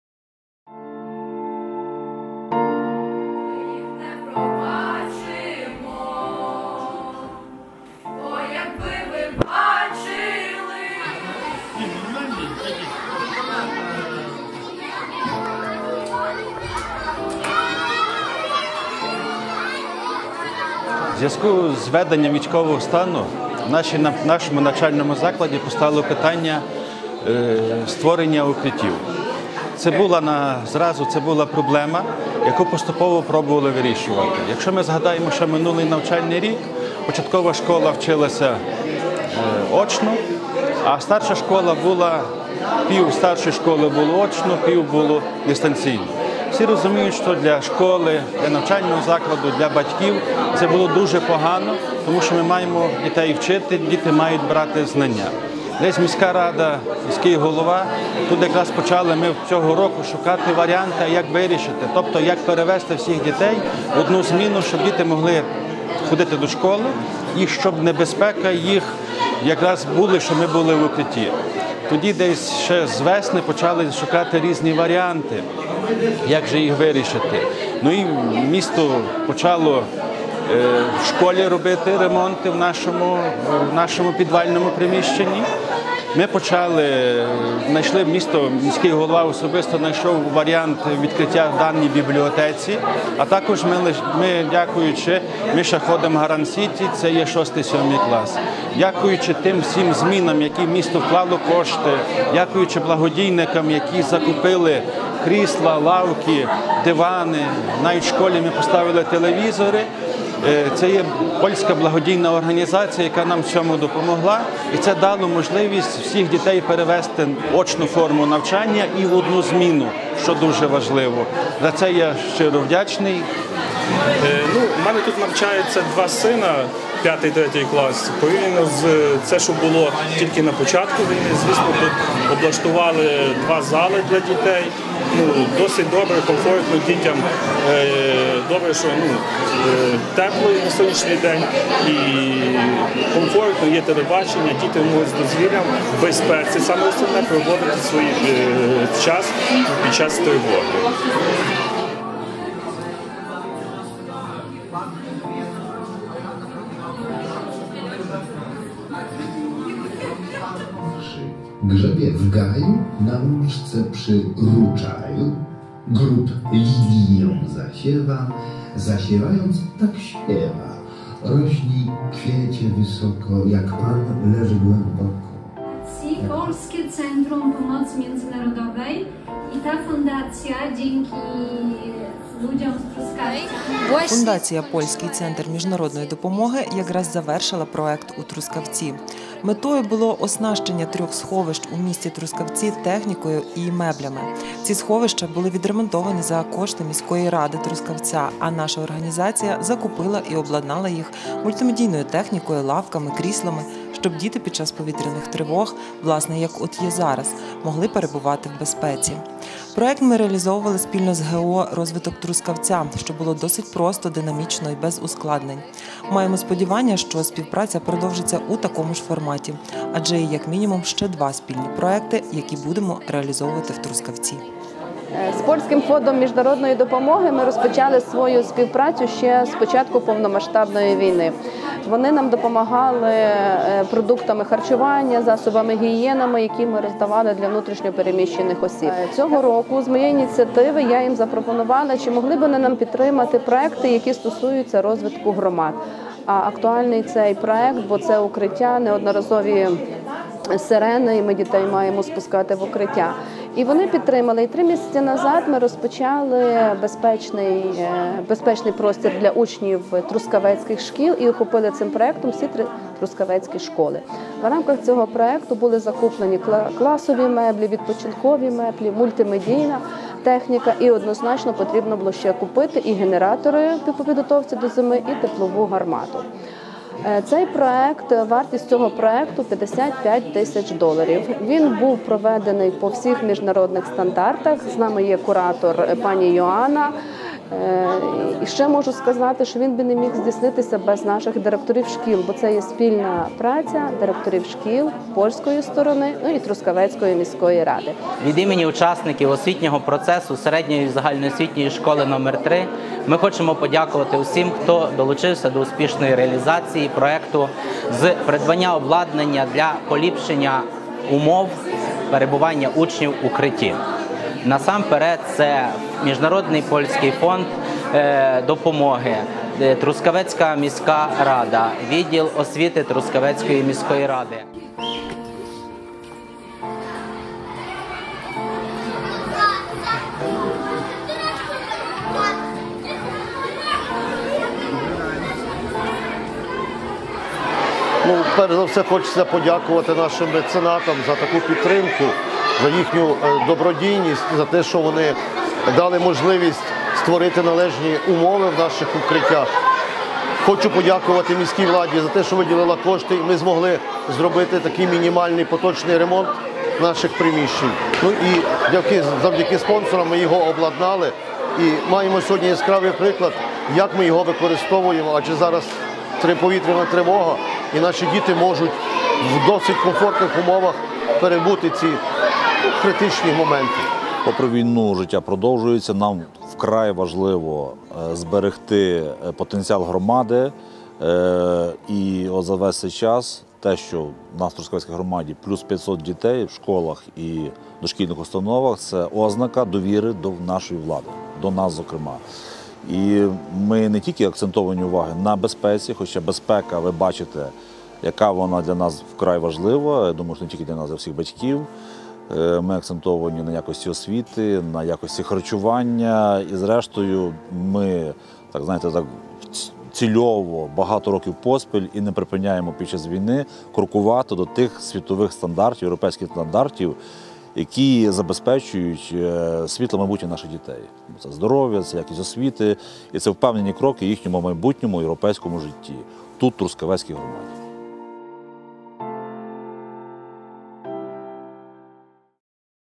TRUSKAVETS: Завершення проєкту по оснащенню укриттів в школах міста Трускавець
Польський центр міжнародної допомоги завершив оснащення трьох шкільних бомбосховищ технікою та меблями у місті Трускавець. Проєкт «Безперервна освіта для дітей (підготовка укриттів)» був реалізований завдяки фінансуванню представництва Тайваню у Польщі.